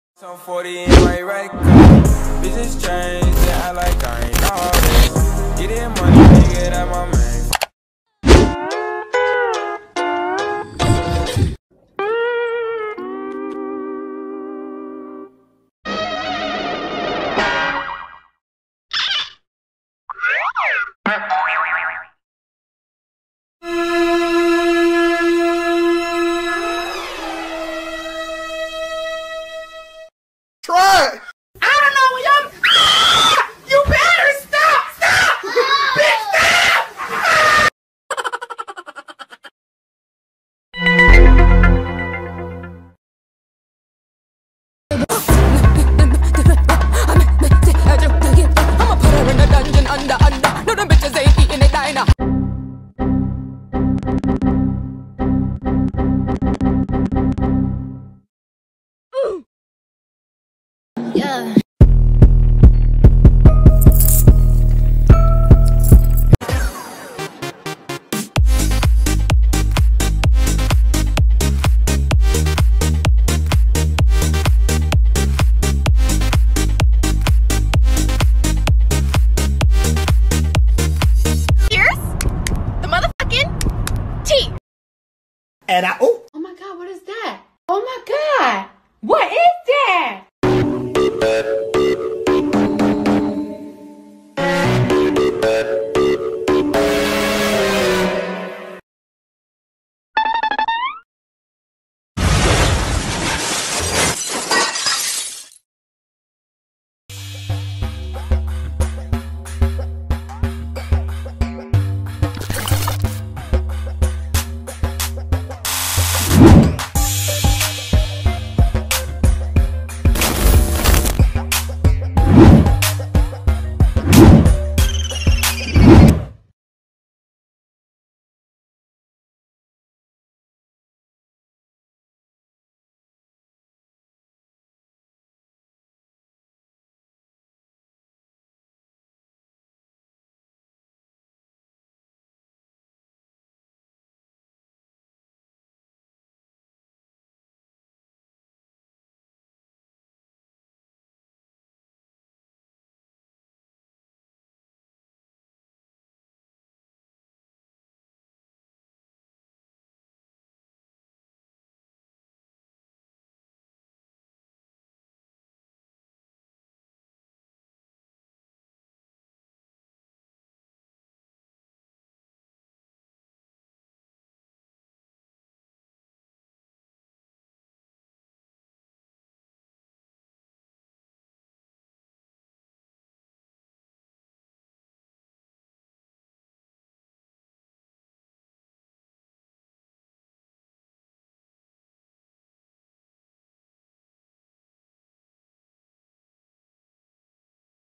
Korean version download in Iphone mobile/ 2020 \ Easy Step
Bht heee ahsaaaaan tareeeqaaa hai

Or ya video korean version k lea hai

Na k pubg mobile k bareey me kuch galt kahnaaa hai

Korean bht hee unique outfit simply mil jaty hai